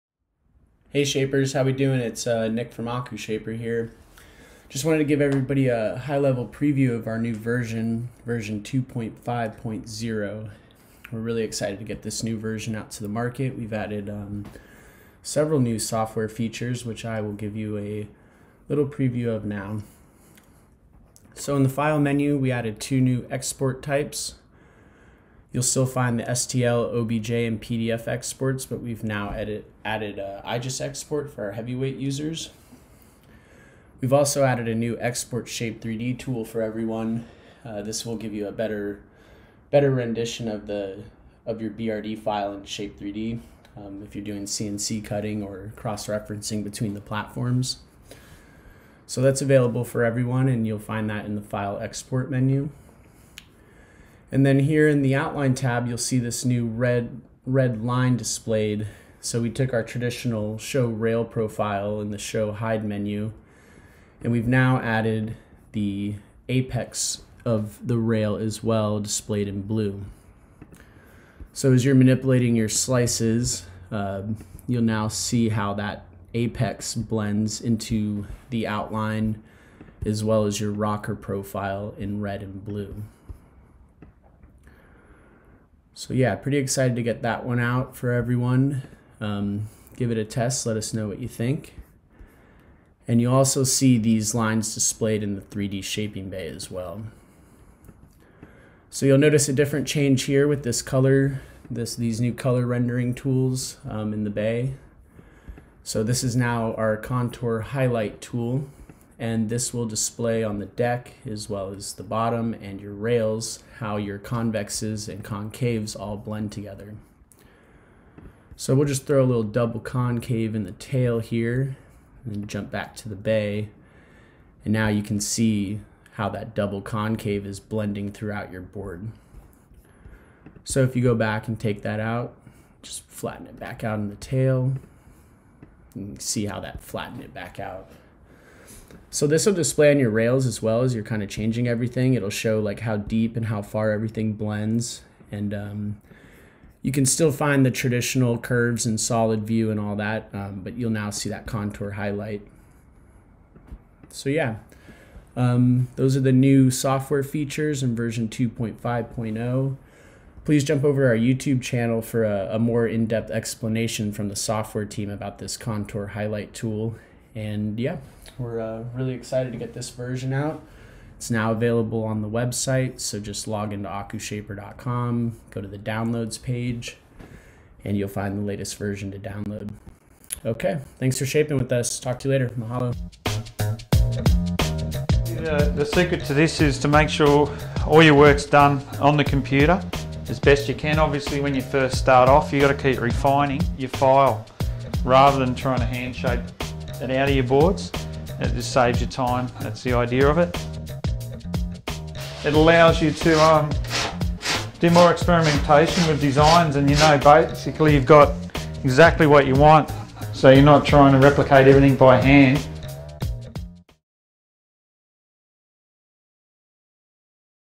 v2.5.0 Release Overview
This video covers the new features that have been added to the latest release of AkuShaper surfboard shaping software. v2.5.0 includes a new IGES export, Shape3d compatibility export, a new rail tuck & apex display tool, and a new Contour Highlight tool in the 3D Shaping Bay. Drop a comment and let us know what you think of the new features and what you want to see next! Mahalo -Team AkuShaper